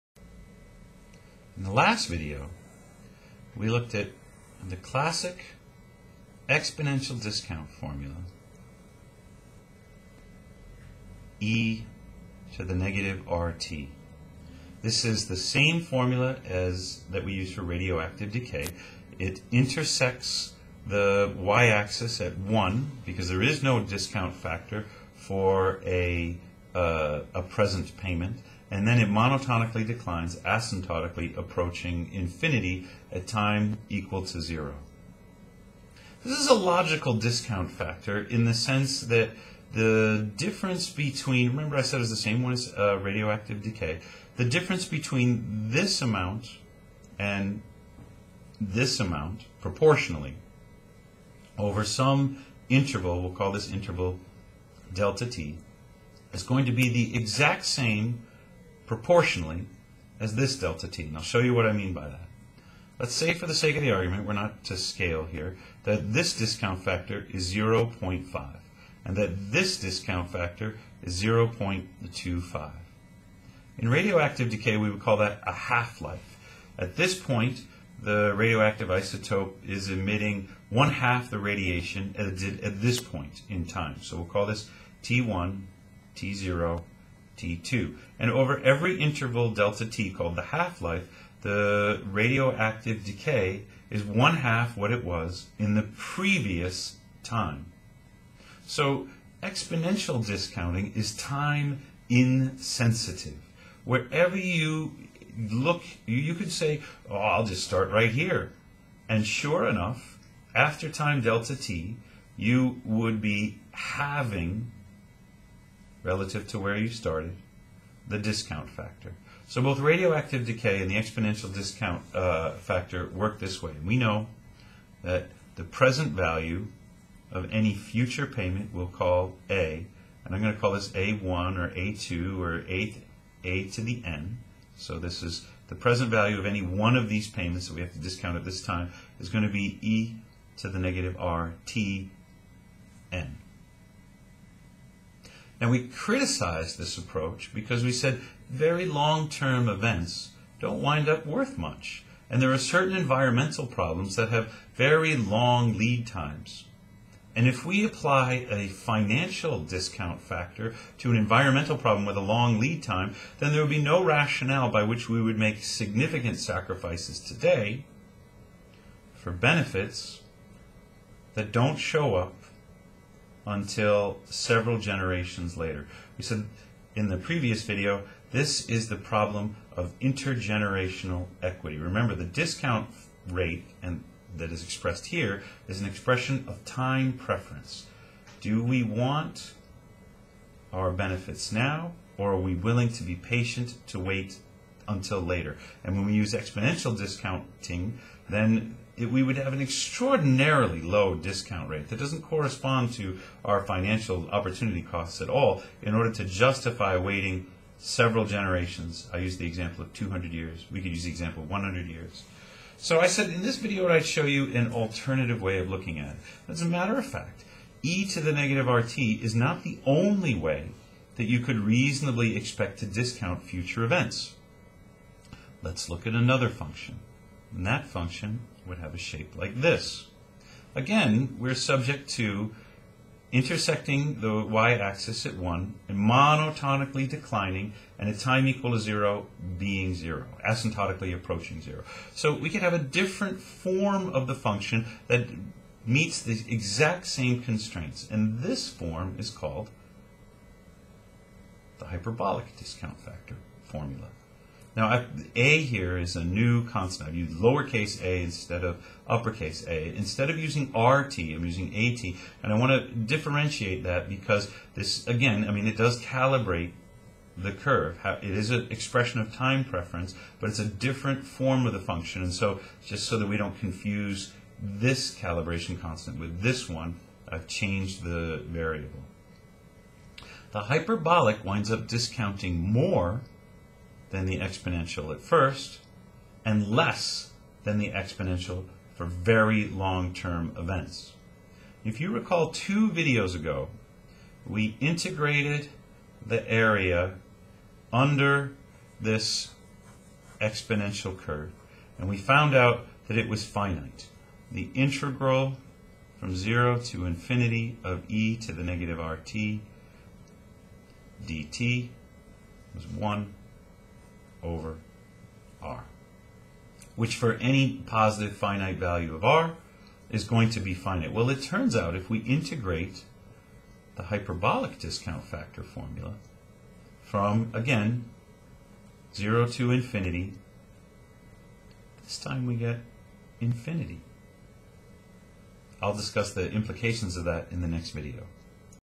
Exponential vs. Hyperbolic Discounting
Both exponential and hyperbolic discount factor functions provide a feasible basis for modeling time preference.  However, hyperbolic values the long-term more than exponential, and the near-term less.